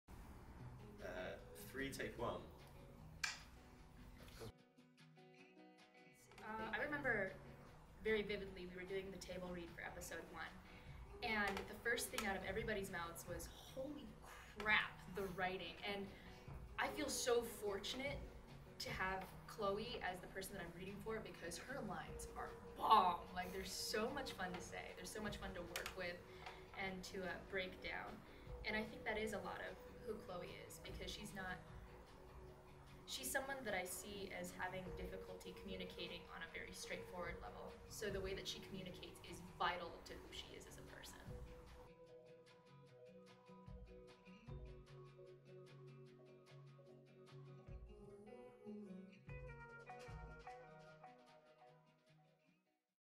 Rhianna DeVries - What is it like recording lines for Chloe?
Here's a little sneak peak behind the scenes of the work that went into creating this game.